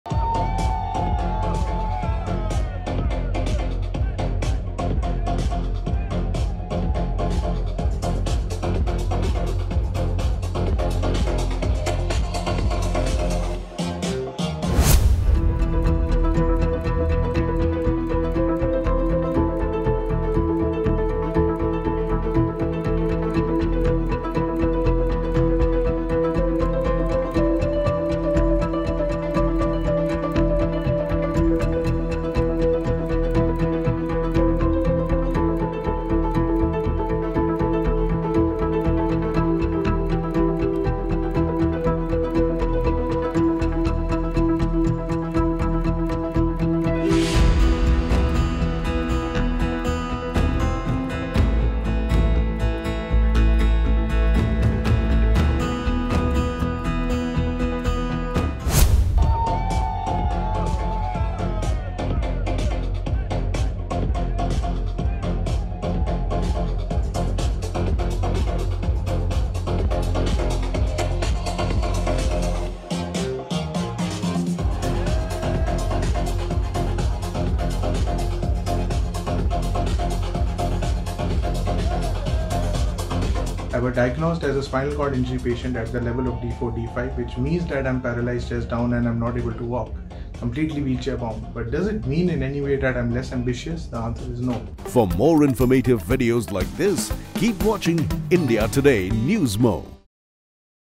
He Is a DJ On A Wheelchair
DJ Aamish aka Varun Khullar is a differently-abled DJ. Watch his journey